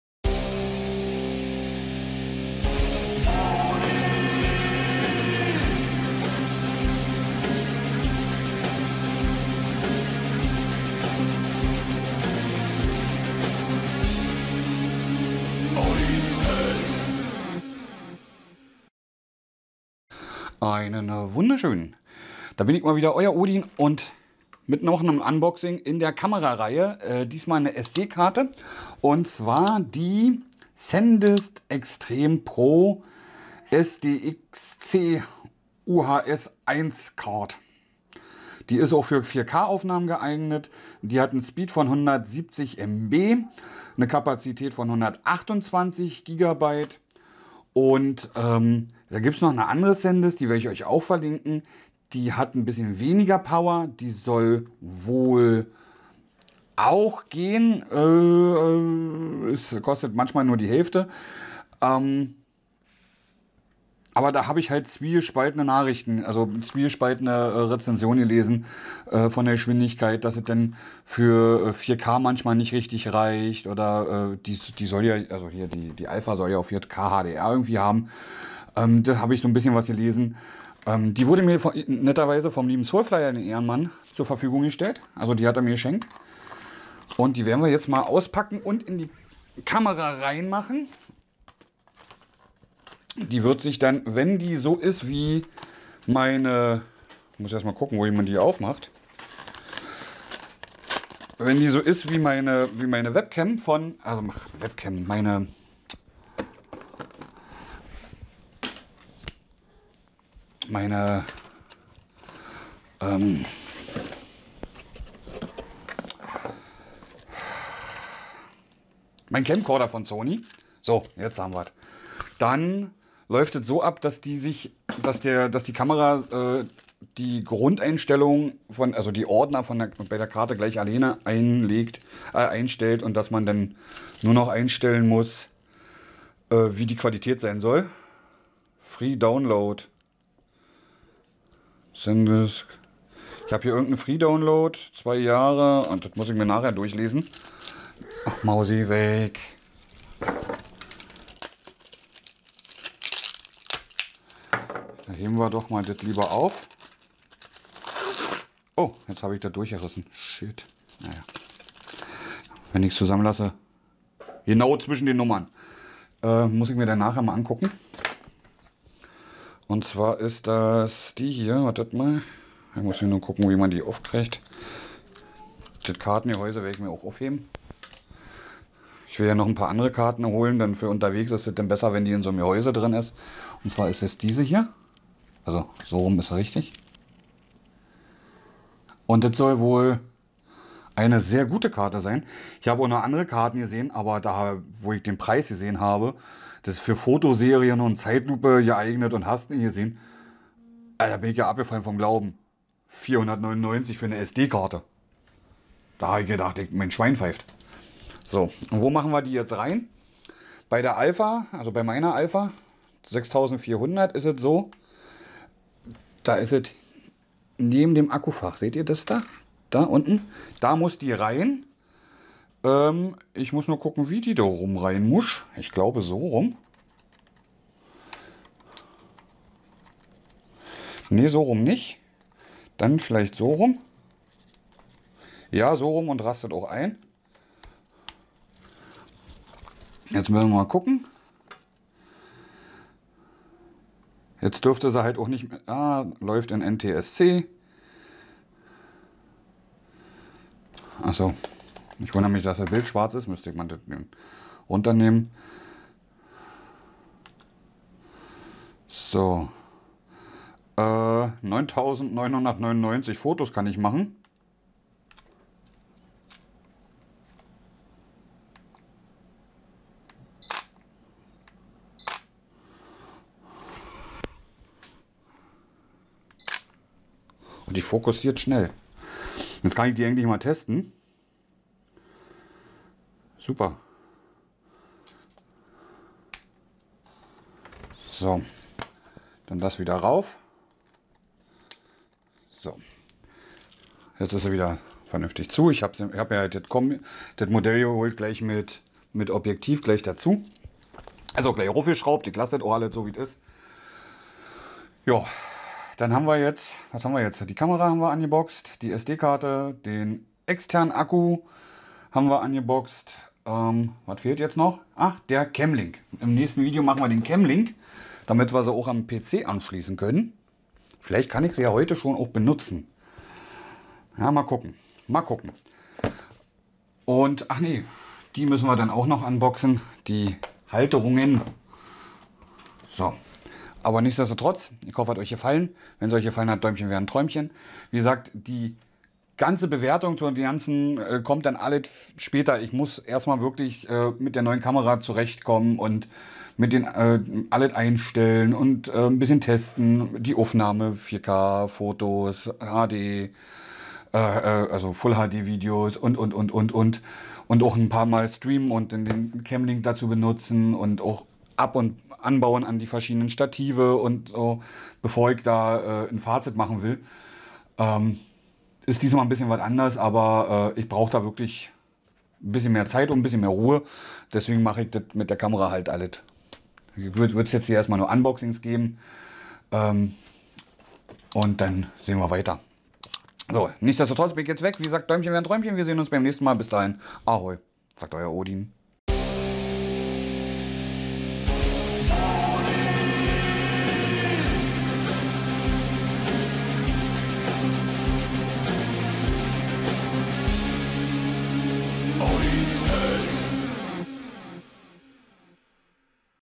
Unboxing | SanDisk Extreme Pro SDXC UHS-I Speicherkarte 128GB
Professionelle Leistung für schnelle Aufnahmen, 4K UHD Videos und beschleunigte App-Performance mit der SanDisk Speicherkarte.
Dank der schnellen Übertragung von bis zu 170 MB/s warten Sie nicht mehr lange, während Ihre Bilder und Videos auf Ihre SD Karte oder von Ihrer SD Karte auf den Computer übertragen werden.
Machen Sie bildverlustfreie Videoaufzeichnungen dank UHS Speed Class 3 (U3) und Video Speed Class 30 (V30) mit der SD Karte für Kamera.
Die SDXC Speicherkarte ist für extreme Bedingungen ausgelegt und ist dazu temperaturbeständig, wasserdicht, stoßfest und röntgensicher.
Lieferumfang: SanDisk Extreme Pro SDXC UHS-I Speicherkarte 128GB (V30, Übertragungsgeschwindigketi 170 MB/s, U3, 4K-UHD-Videos, temperaturbeständig ) 30 Jahre Garantie
👍🖖🤟Mein Equipment👍🖖🤟
Ihr habt Bock mit mir zu zocken oder zu texten? Kein Problem:
PSN: odin_ase
Steam: odin_ase